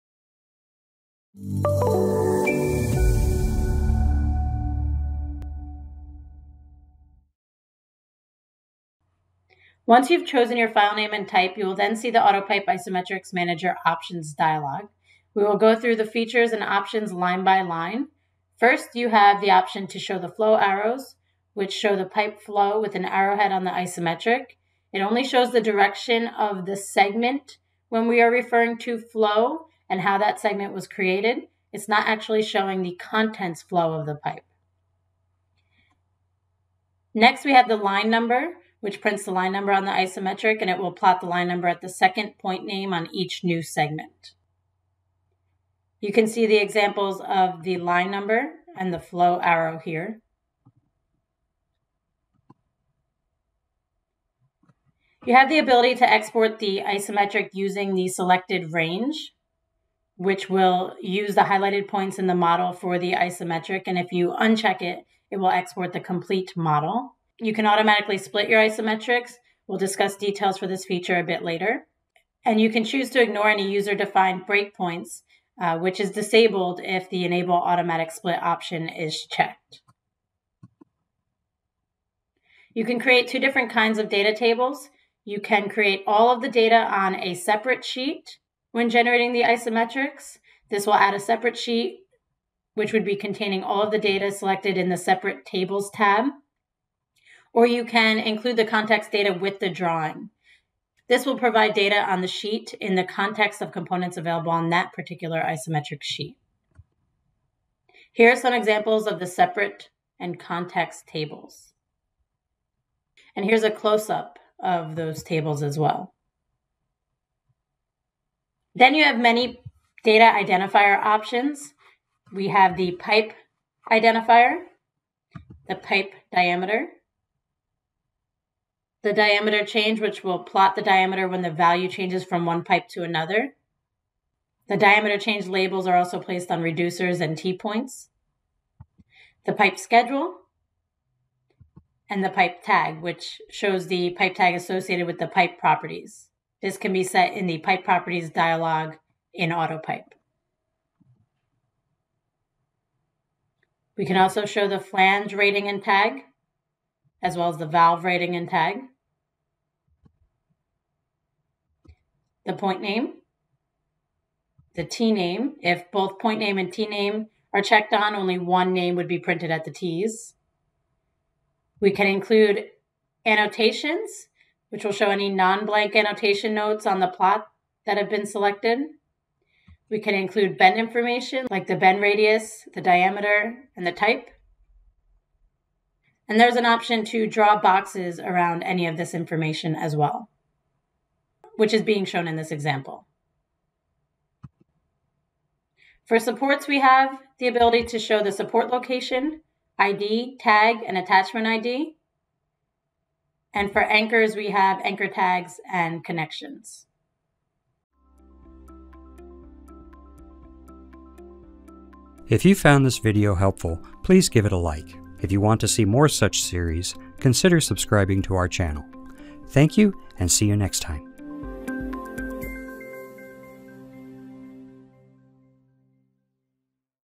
Stress Isometric Creation - Presentation - Plot Options
Reviewing the available plot options when creating a stress isometric in AutoPIPE.